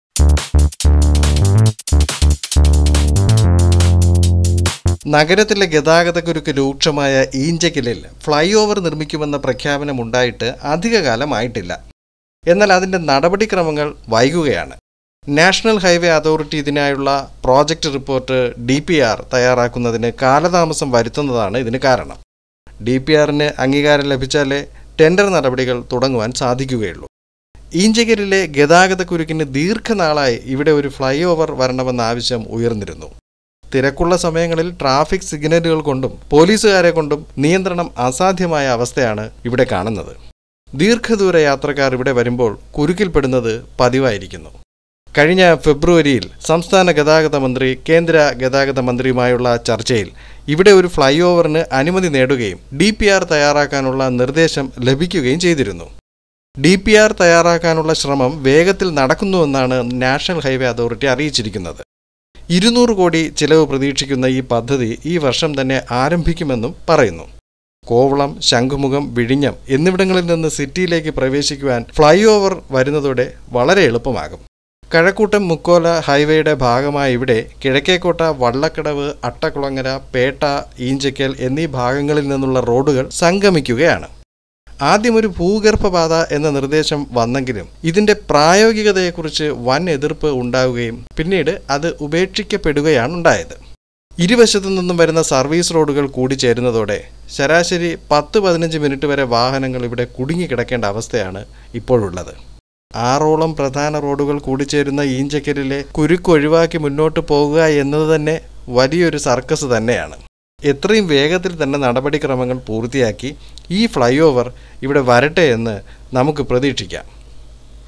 flyover at Enchakkal on NH-66 bypass is likely to be delayed|thiruvananthapuram
The construction of the flyover at Enchakkal on NH-66 bypass is likely to be delayed as the regional office of the National Highways Authority of India (NHAI) is yet to submit to its headquarters the estimate prepared as part of a detailed project report (DPR). The tender for the project can be floated only after the approval is received, said an NHAI official. With the area witnessing acute traffic congestion, a flyover at Enchakkal junction has been a longstanding demand. The traffic congestion, especially during peak hours, leads to a long queue of vehicles. The arrangement with traffic
police personnel and signals can’t go on forever. Also, long-distance vehicles waiting at the junction on a national highway for long is unacceptable. In February, Transport Minister Antony Raju had called on Union Road Transport Minister Nitin Gadkari in New Delhi after getting in-principle approval for the project. Subsequently, the Union minister had directed NHAI to prepare the DPR in three months. Meanwhile, a senior NHAI official said they will find a solution at the earliest, with the work on DPR preparation progressing.We have prepared an estimate for the project which will be included in the DPR. It will soon be submitted to the headquarters for approval. Once the proposal including the design gets approval, we will go ahead with floating tenders. The whole process will take at least two months. hopefully, the preliminary work can be started within three months,” said P Pradeep, project director, NHAI. The work on the Rs 200-crore project is expected to begin this year itself. The Enchakkal flyover is envisaged to facilitate the smooth entry of vehicles from Kovalam, Shankhumukham and Vizhinjam to the city, besides easing the congestion at the junction. Located on the busy Kazhakootam-Mukkola national highway stretch, the junction has roads from East Fort, Vallakadavu, Attakulangara and Pettah-Enchakkal converging. Earlier, the NHAI had proposed an underpass there but the plan was scrapped after traders opposed it. Since the Chackai flyover was opened to the public, the traffic congestion at Enchakkal has become a cause for concern to commuters. The congestion primarily happens on the service road from Chackai to Kovalam, where vehicles remain stuck for 10 to 15 minutes. That affects the movement of vehicles on other roads too. The local residents and traders have welcomed the decision to construct a flyover.